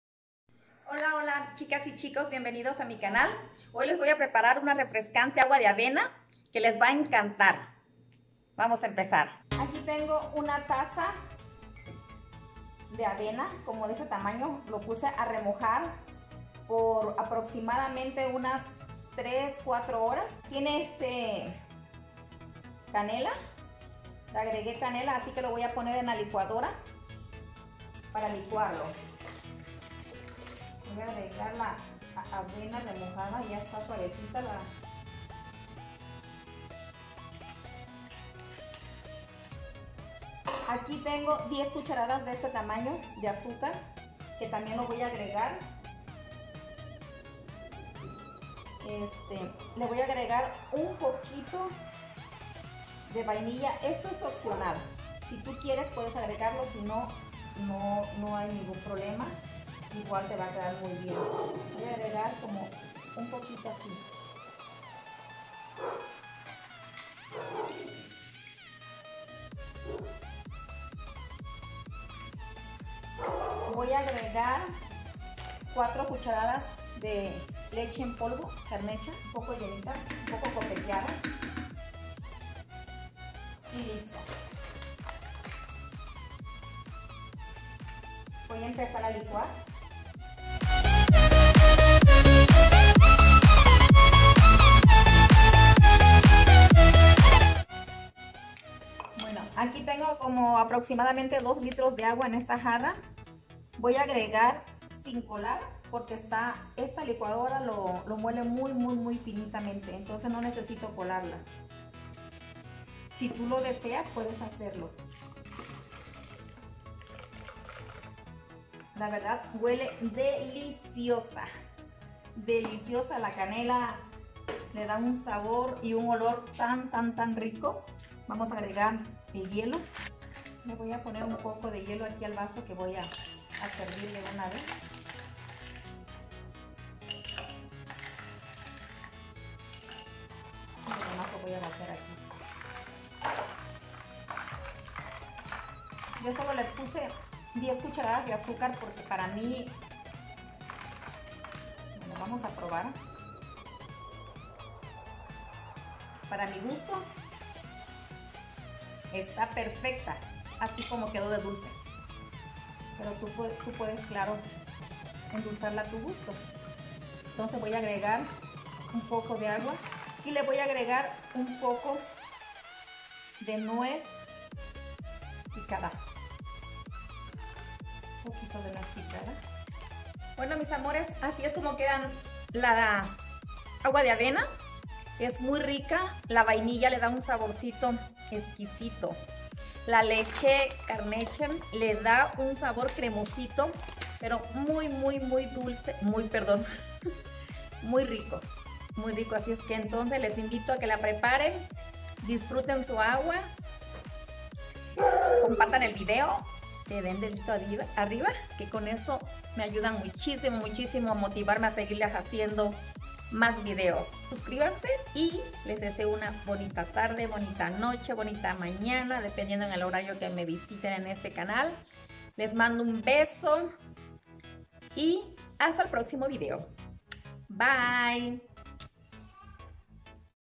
AGUA DE AVENA CON NUEZ
1taza de avena, 4 cucharadas de leche carnation en polvo, rajas de canela al gusto, azúcar al gusto, 1/2 cucharadita de vainilla (opcional) 2 litros de agua y hielo al gusto